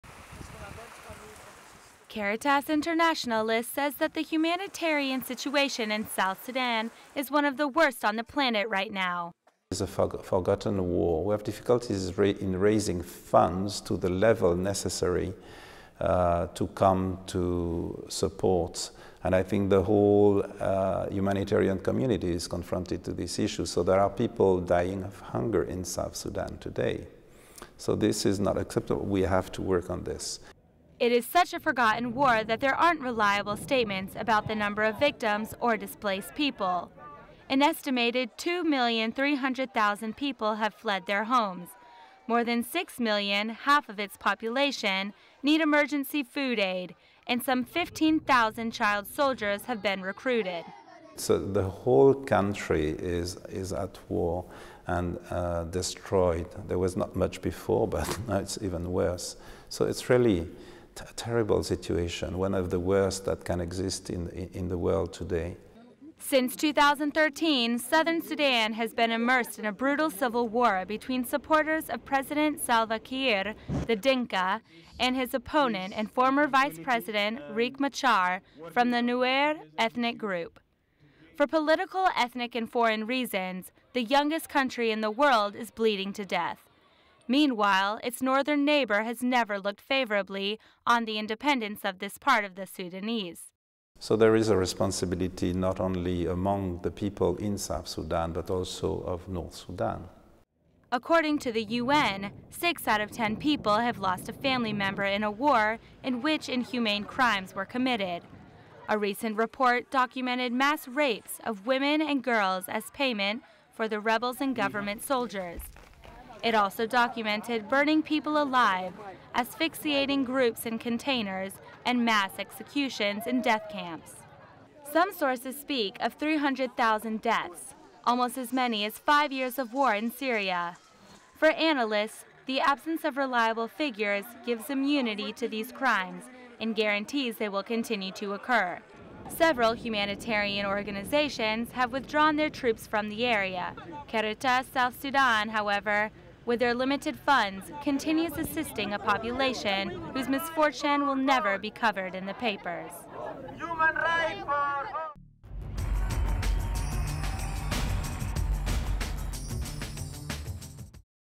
Caritas: Humanitarian crisis in South Sudan is one of the most terrible in the world
May 10, 2016. At war since 2013, the UN speaks of the mass killing and raping of women and girls.